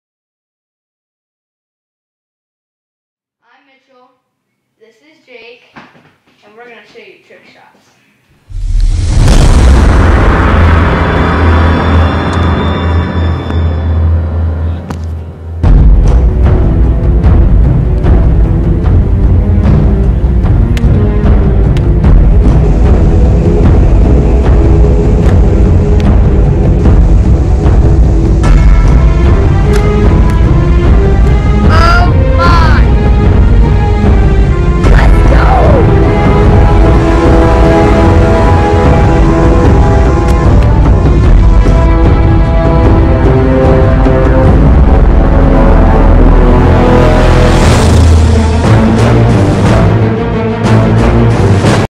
Trick Shot City 2012
Jake shooting trick shots at Mitchell's